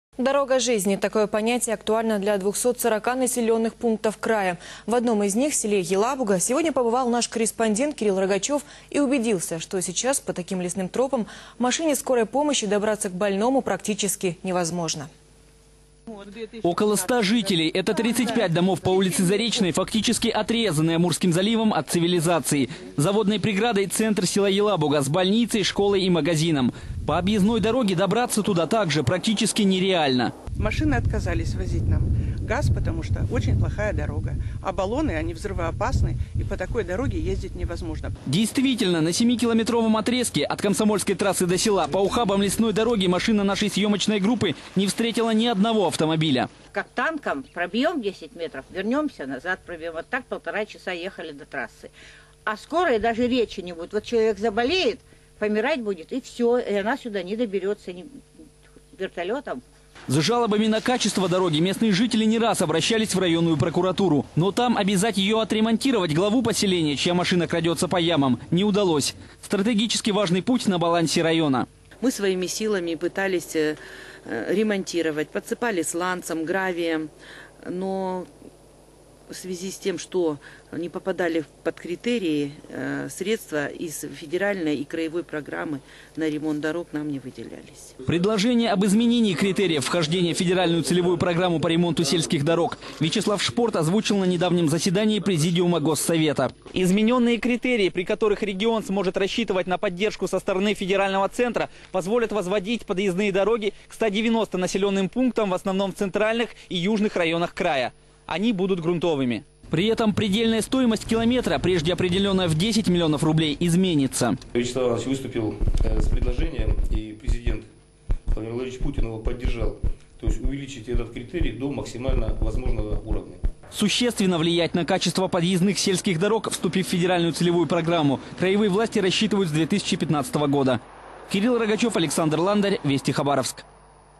Вести-Хабаровск. Разбитые сельские дороги
"Дорога жизни". Такое понятие актуально для 240 населенных пунктов края. В одном из них - в селе Елабуга - сегодня побывал наш корреспондент Кирилл Рогачёв и убедился, что сейчас по таким лесным тропам машине "скорой помощи" добраться к больному практически невозможно.